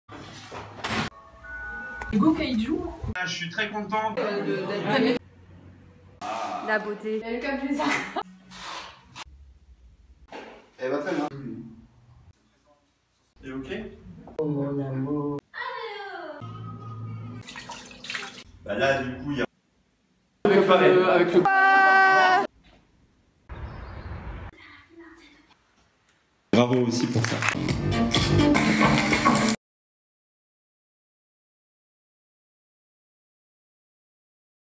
LA SECONDE PASTEL MARS 2018
Revivez tous les événements du Studio PASTEL à Reims en quelques secondes !
Profitez-en pour voir ou revoir les vidéos de la chaîne YouTube de la Compagnie PASTEL.